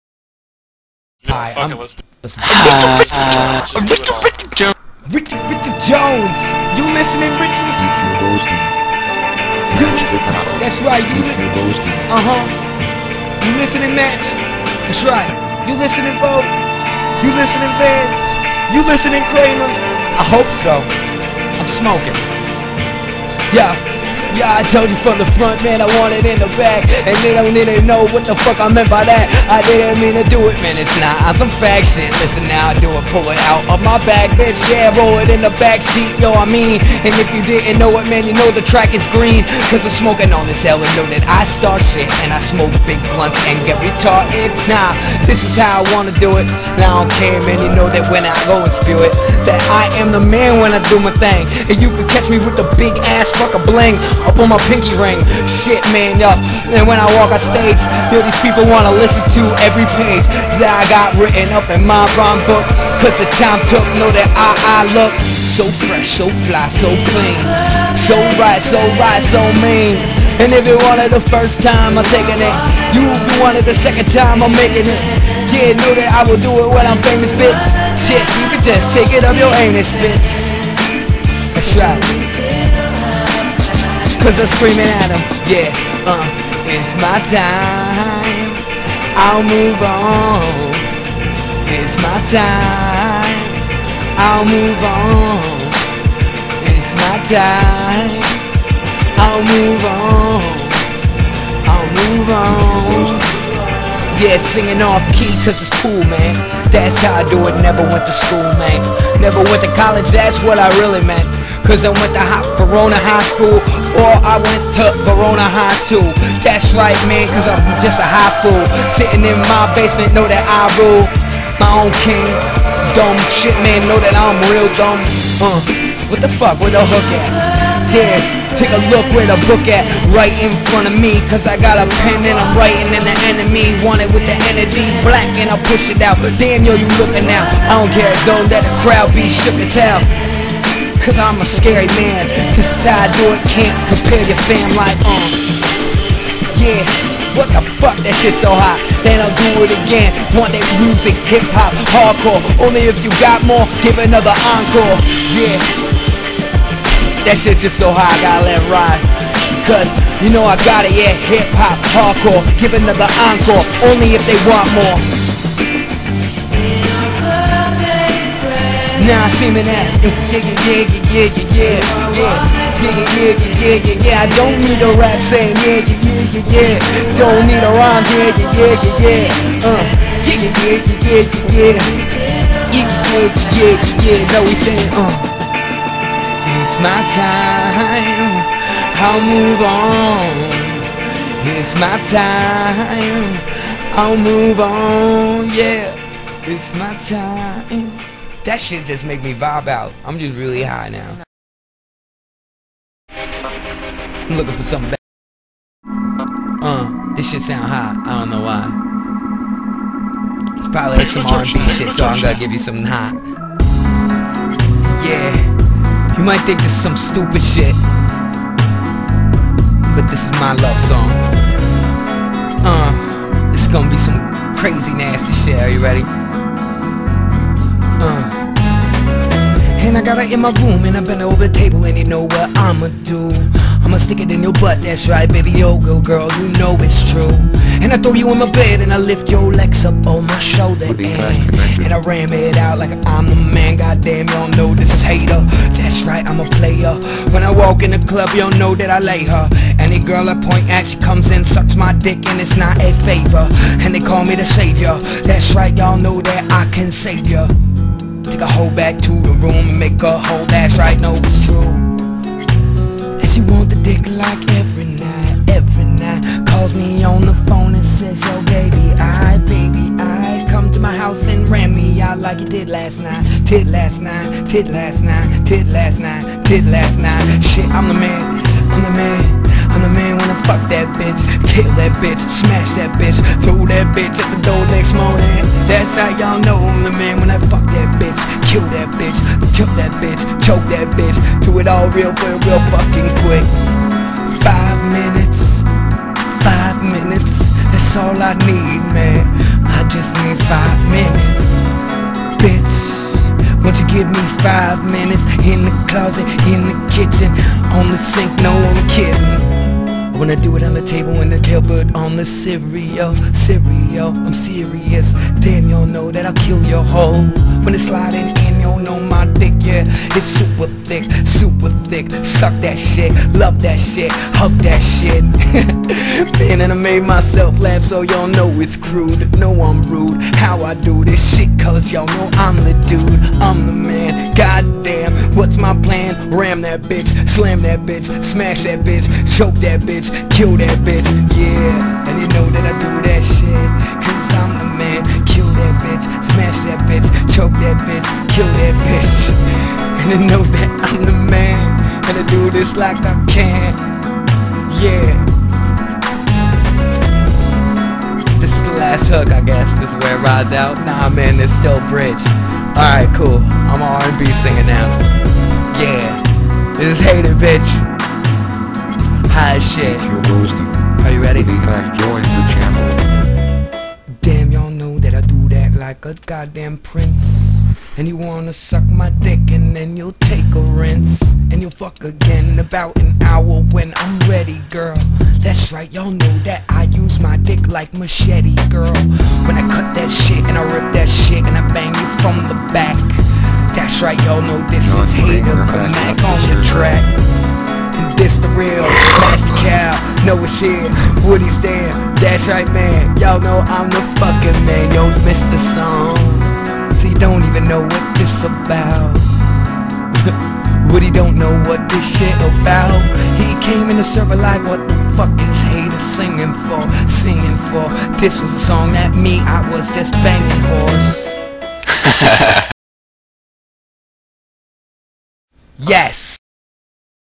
HaTeR Live Mixtape EQ hosting 2 track demo
1/23/2009 HaTeR got down in the studio with myself and Rich Jones and freestyled over a couple beats he has been romancing in the studio. An Event will follow once we are assembled.

-Match Tical